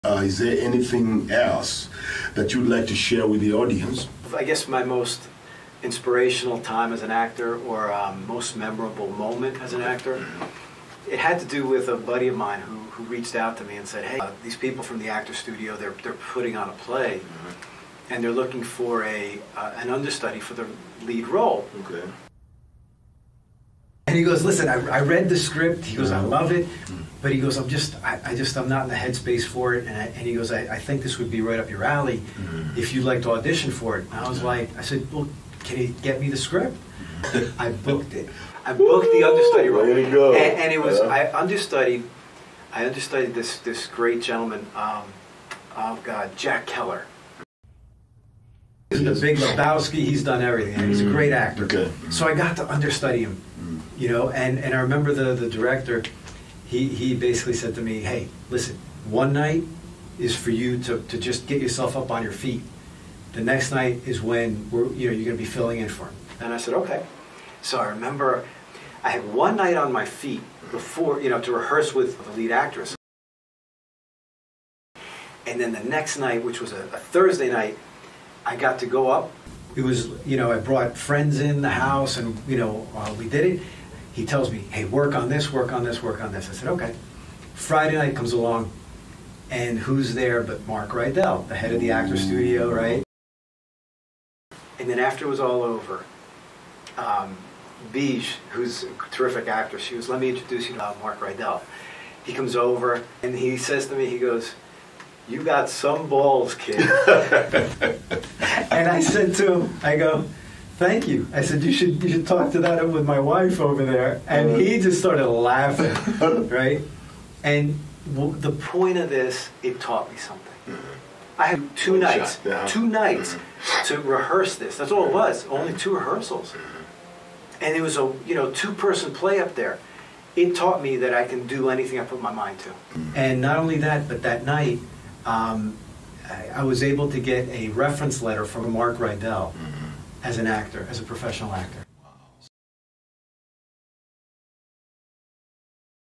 George Vincent Shares Most Memorable Acting Moment
In this clip I share my most memorable acting moment while being interviewed by my dear friend and fellow actor Benjamin Onyango on his YouTube Show - Frank Talk on AMR Films YouTube Channel.

I had an amazing experience auditioning for and performing with the Actors Studio in Los Angeles for the West Coast Premiere of Arthur Miller's "Some Kind of Love Story."  I understudied the male lead role performed by the talented Jack Kehler - known for his roles on, The Big Lebowski, Men in Black II & more. That night in the audience was Academy Award-Nominated director - Mark Rydell  and after the play he had some inspiring words for me which led to a professional actor reference letter for me.  Enjoy the clip!